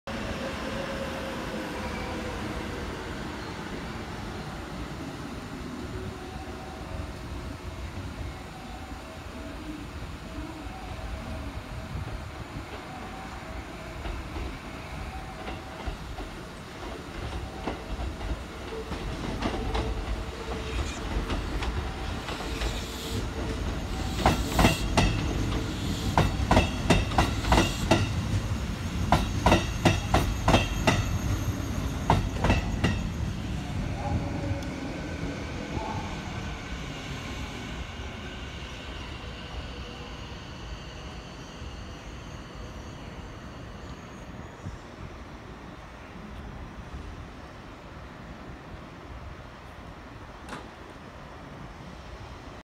山陽6000系6001F普通東二見行き 山陽明石駅入線
山陽6000系の1次車です。
山陽明石駅で直通特急待ちありです。